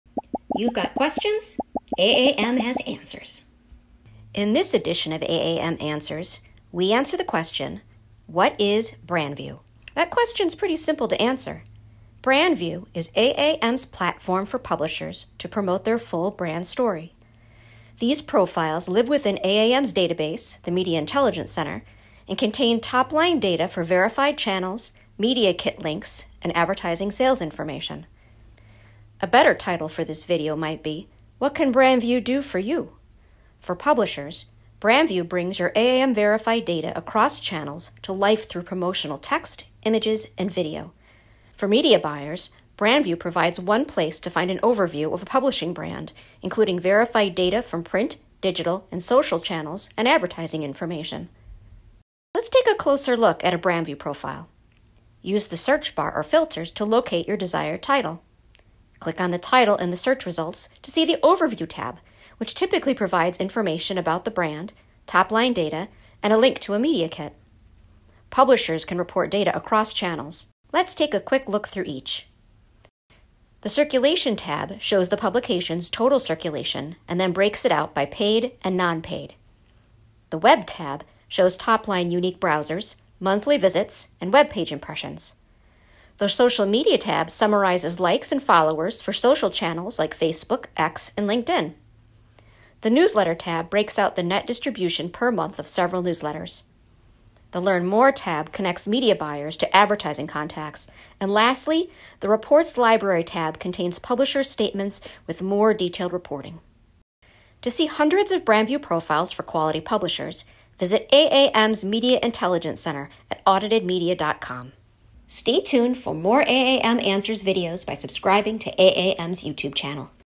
AAM Answers: What is Brand View?
Have you seen AAM Brand View? It’s AAM’s platform for publishers to promote their full brand story via top-line data, images, video and more. In this edition of AAM Answers, you’ll find out how Brand View can help you, whether you’re a publisher or media buyer.